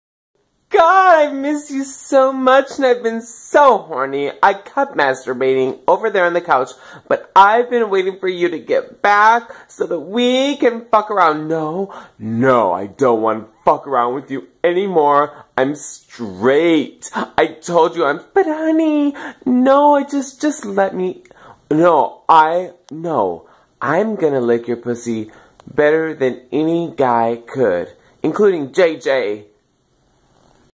SoaParody DAYS Eve comes on to Paige
An inappropriate mother/daughter interaction